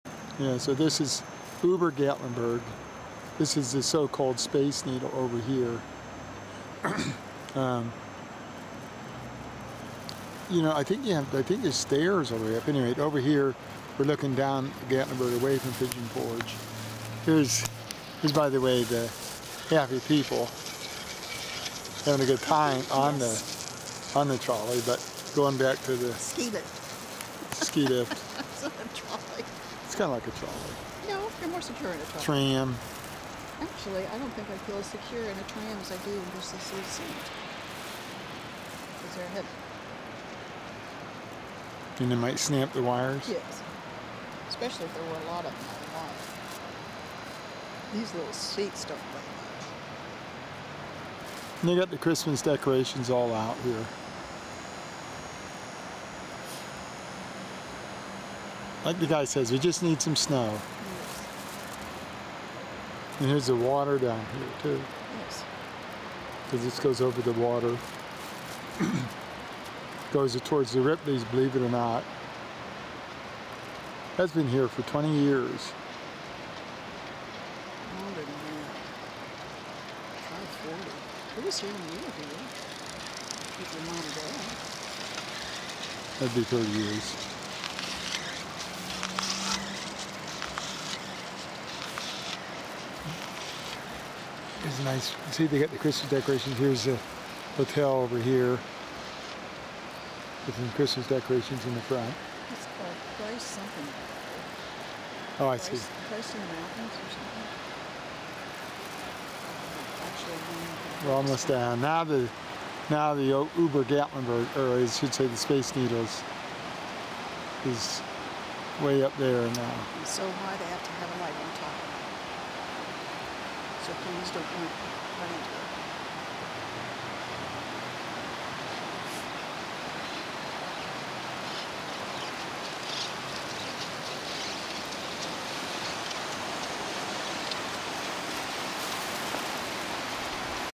Gatlinberg sky lift at night
The Gatlinburg Sky Lift is the best way to view Gatlinburg and the Smoky Mountains. On a clear day, you see almost forever. The Sky Lift gently glides over the Little Pigeon River and then climbs to the overlook at an elevation of

1,800 feet. The vantage point is unparalleled. It's Gatlinburg's #1 scenic overlook. Enjoy our Gift Shop, Picnic Area, and Snack Shop on the top.